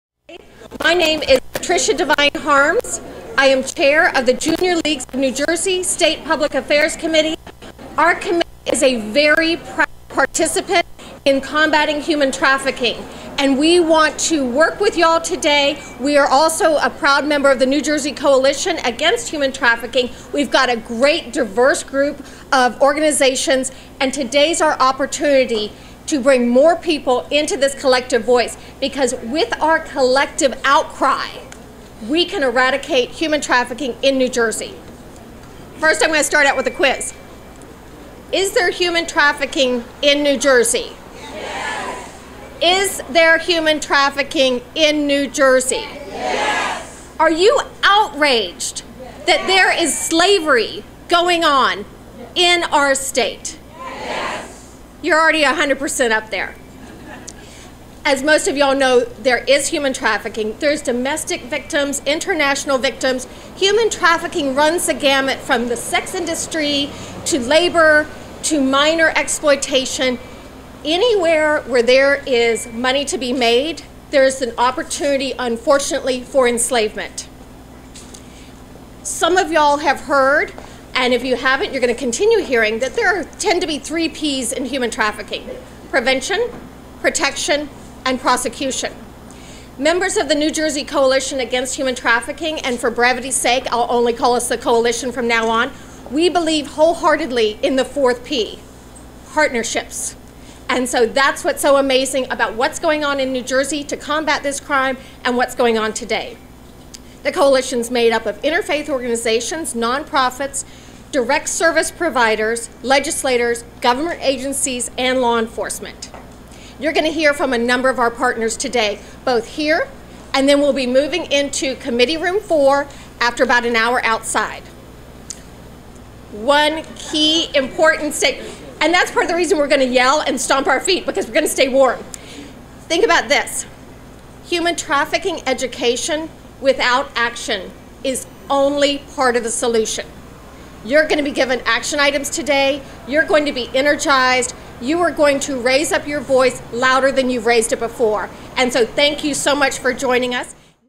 Patricia Devine Harms
Patricia Devine Harms of the Junior Leagues of New Jersey introduces the Human Trafficking Rally